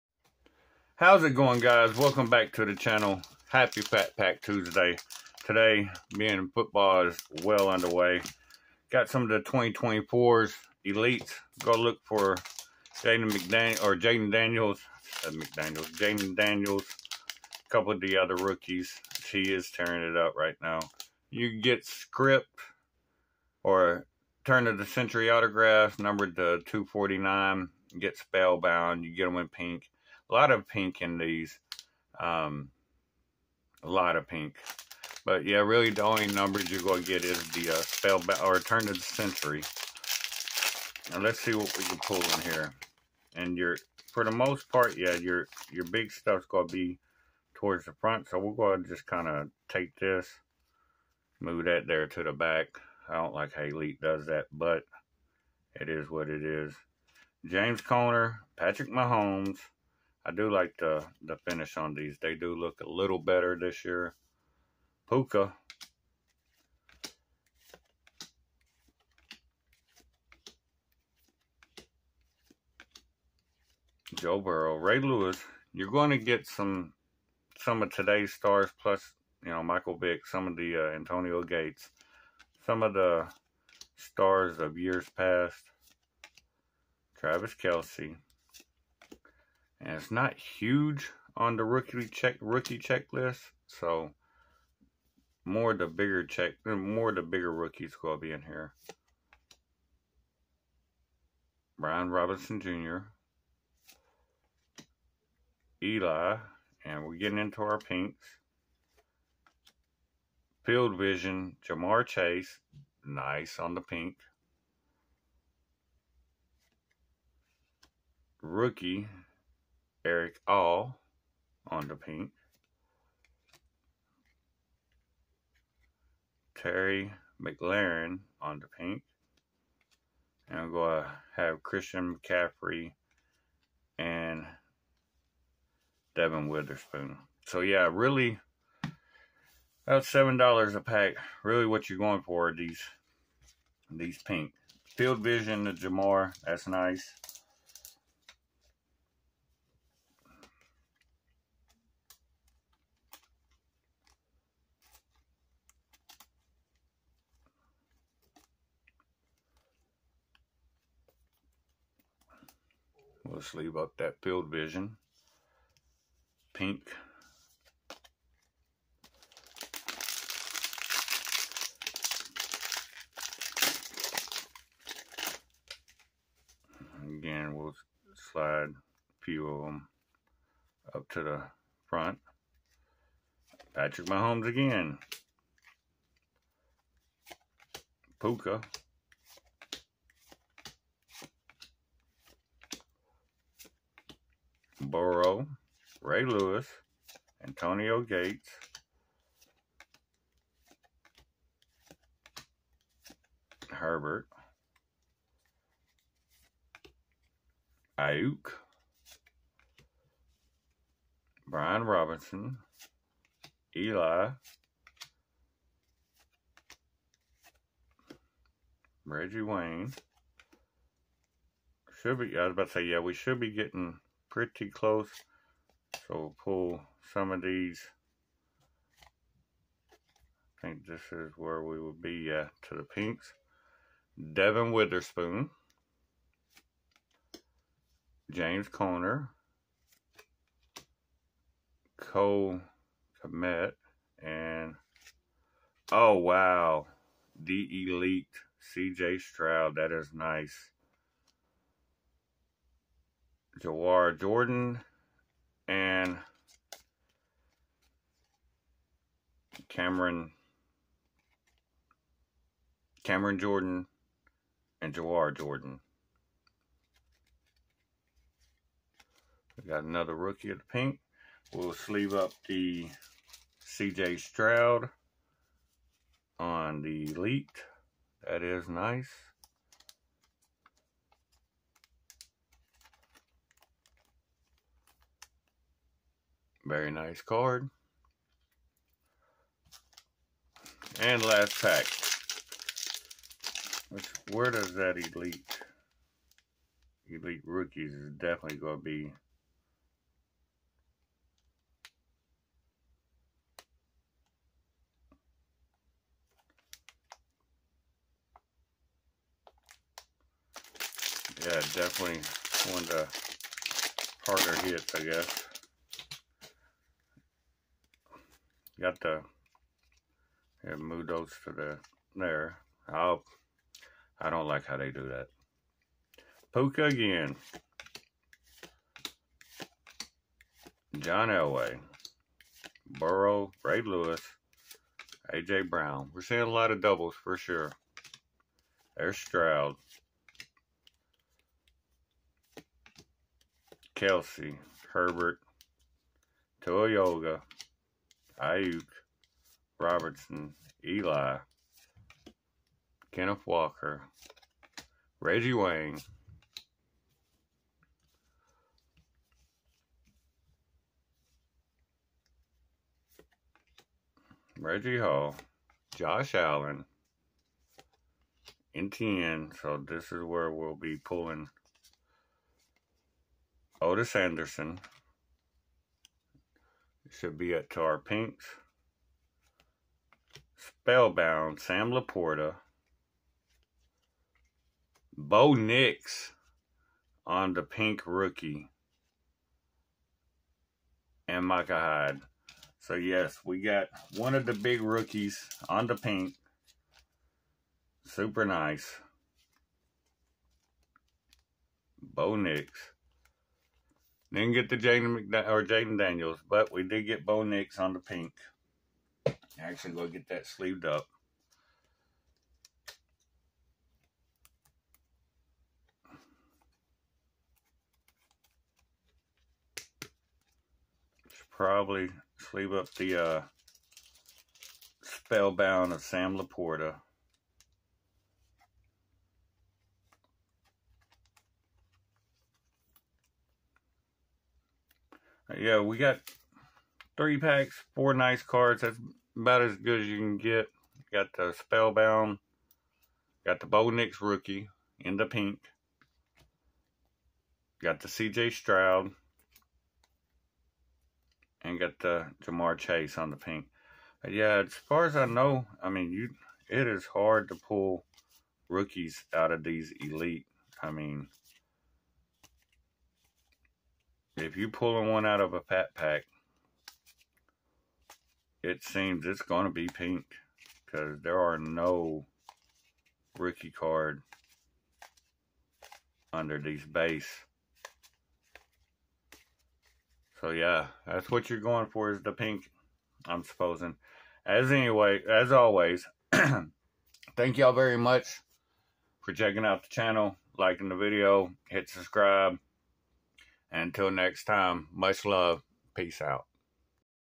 FAT PACK TUESDAY Huge Pink Rookie QB... Elite is ....Well Elite
Panini Donruss Elite football 2024 fat Pack opening and review